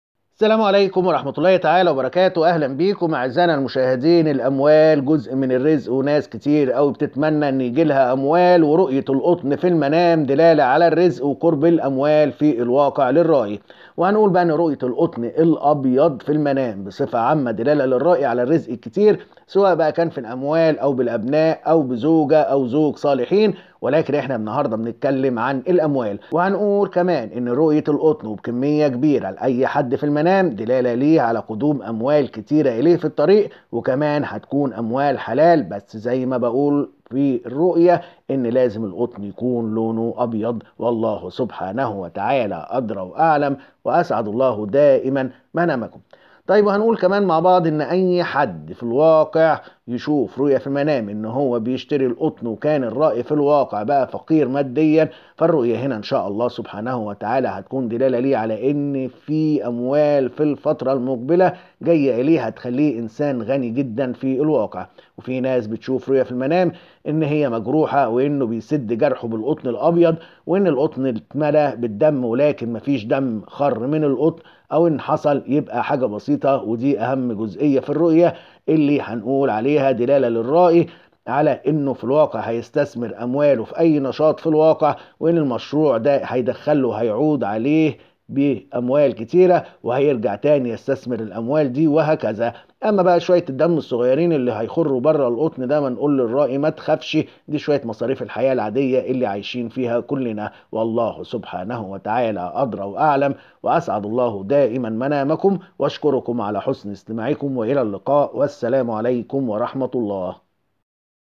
أقترب المال مع رؤية القطن
تفسير رؤية القطن في المنام
رؤية القطن في المنام التي تدل للرائي على قرب قدوم أموال في الطريق
متى تكون رؤية القطن في المنام دلالة للرائي على قرب قدوم أموال في الطريق إليه مع التوضيح
متى تكون رؤية القطن في المنام دلالة للرائي على قرب قدوم أموال حلال في الواقع
كيف يعرف الرائي أنه سيحصل على أموال كثيرة في الواقع مع رؤية القطن في المنام بتفاصيل معينة مع تفسير الرؤية
الإنسان الفقير مادياً في الواقع متى تكون رؤية القطن في المنام دلالة له على قرب الحصول على أموال تخرجه من الفقر
رؤية إستخدام القطن للجروح في المنام و علاقتها بقرب قدوم أموال و أيضاً استثمار هذه الأموال في الواقع